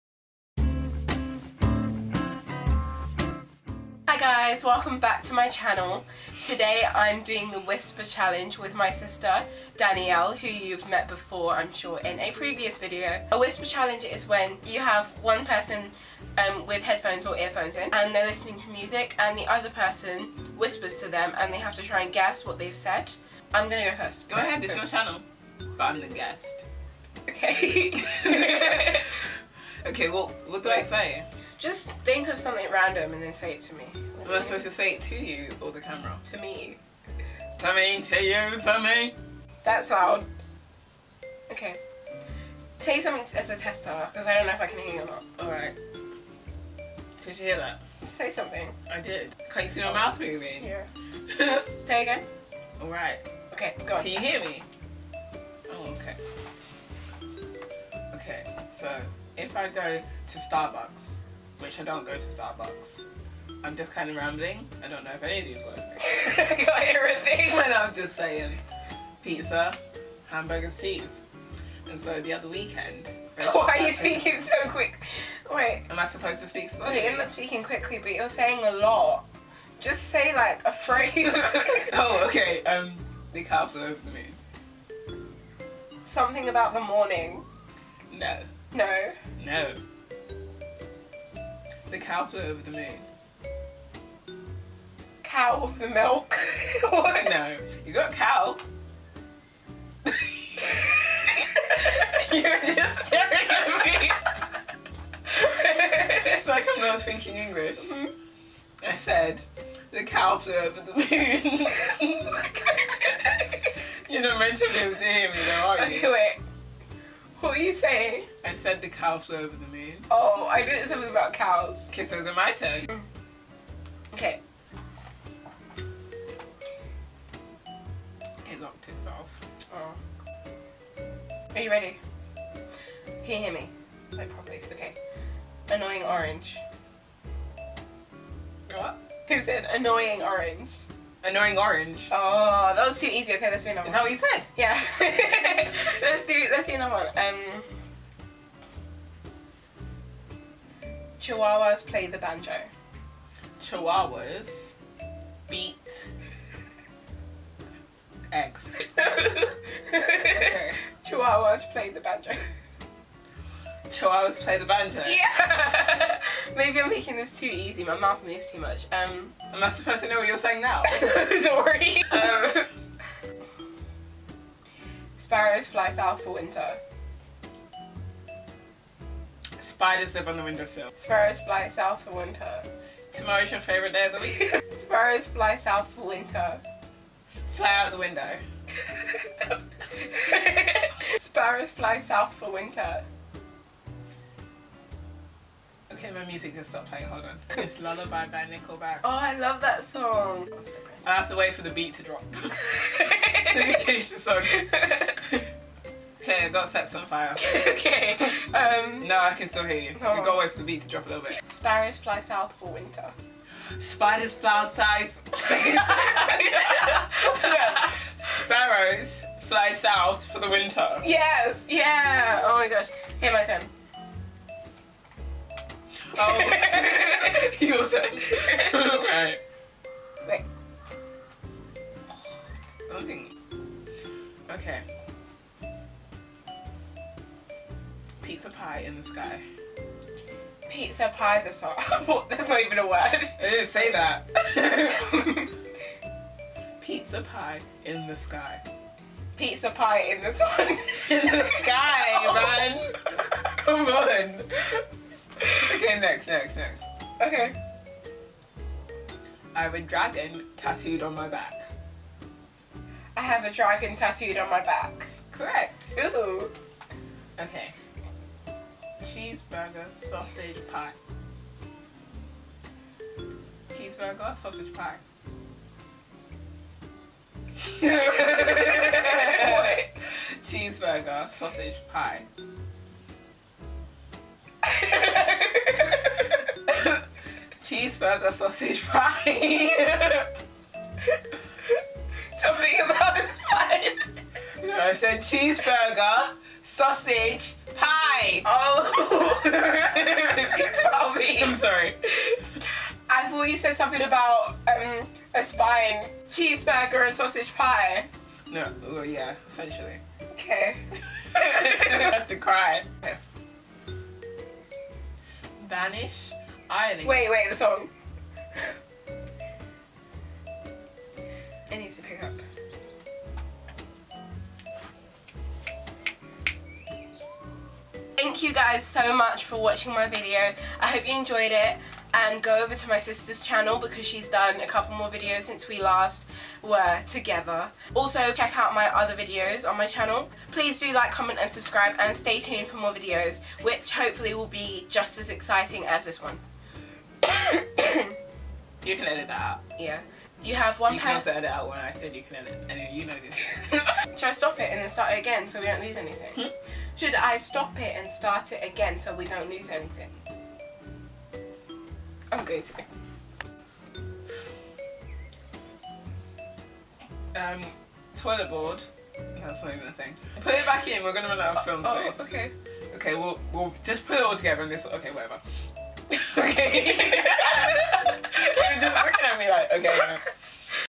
The Whisper Challenge! || The Beauty Cannon
I'm back with a new challenge video, featuring my sister! I hope you guys like it.